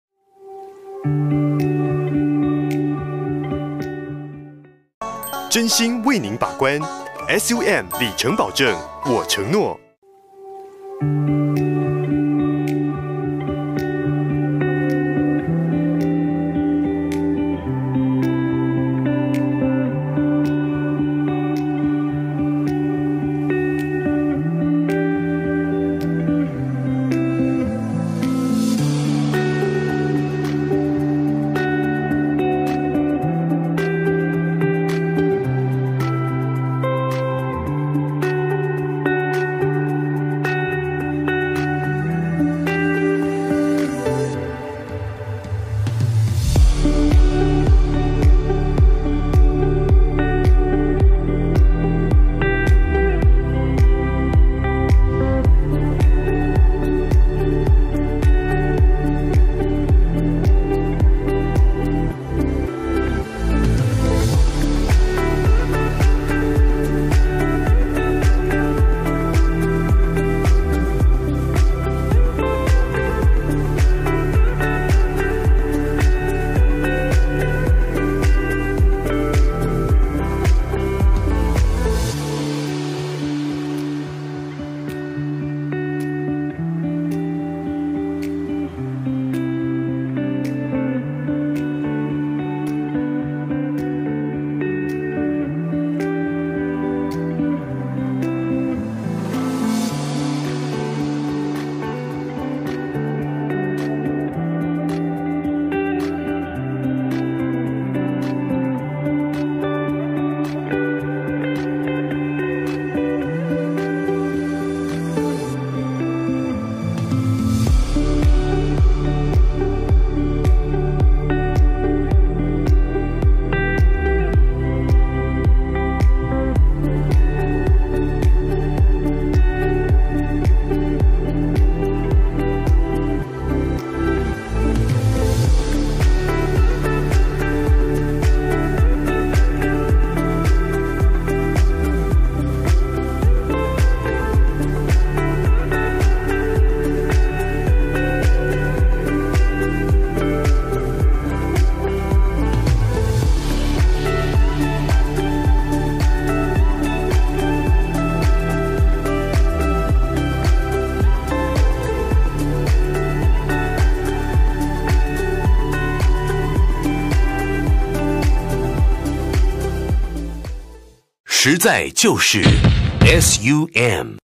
已收訂【SUM中古車】福斯 TIGUAN 2018年ACC跟車.原鈑件.里程保證.第三方認證.可全額貸款.強力過件( 祥順汽車)
車商資訊：SUM祥順汽車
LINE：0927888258
聯絡人：劉淙欽
車輛資訊：2018年福斯 TIGUAN
車輛售價：49.8萬
車輛所在：高雄市左營區重立路390號
「夏日購車季🚗 FUN心出趣玩」
🎁會員購買YES認證車、並啟動保固，週週抽SUM噴霧式滅火劑及養車金!